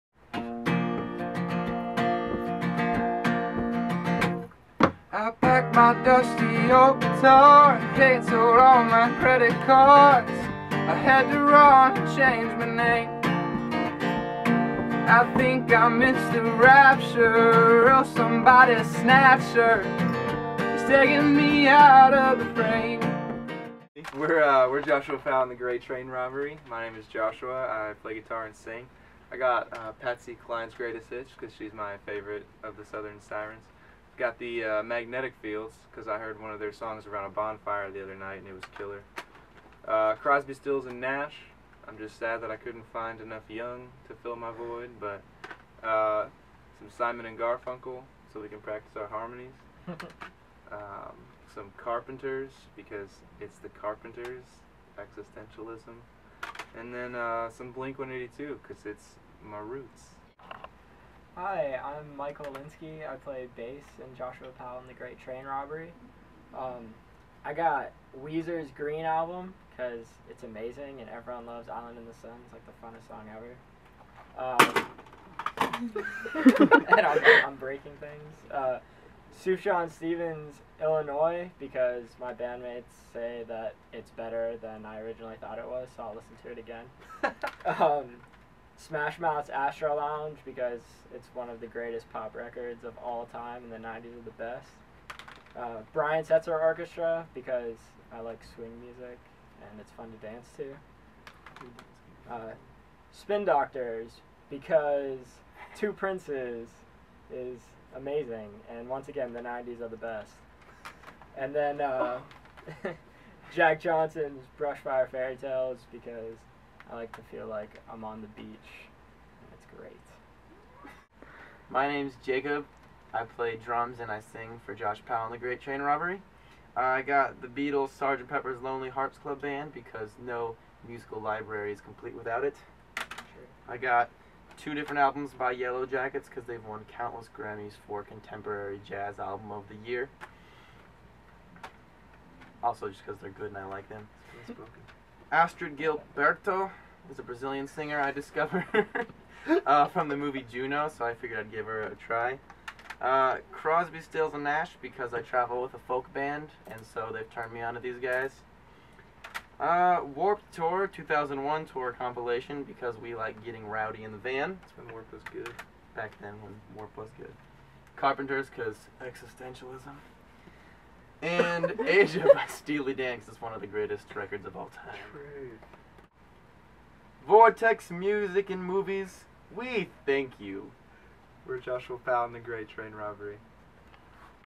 Joshua Powell and The Great Train Robbery Shops At Vortex
Joshua Powell and The Great Train Robbery stop by the store and share what they got.

Joshua Powell Links:
JoshuaPowellMusic.com
Facebook.com/JoshuaPowellMusic
Youtube.com/User/JPTrainRobbery

Shot at Vortex Music & Movies
12525 Totem Lake Blvd NE, Kirkland, WA 98034
VortexMusicMovies.com
Facebook.com/VortexMusicMovies
Twitter.com/VortexMnM